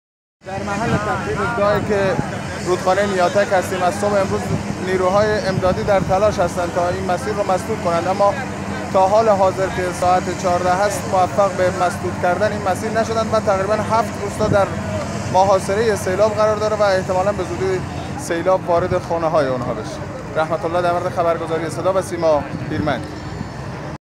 🔴 گزارشی از سیل در شهرستان هیرمند در استان سیستان و بلوچستان
🔴 گزارشی از سیل در شهرستان هیرمند در استان سیستان و بلوچستان                                             هفت روستا در محاصره سیلاب قرار گرفته و تلاش برای مهار سیلاب ادامه دارد.                                            یکشنبه ۱ اردیبهشت ۹۸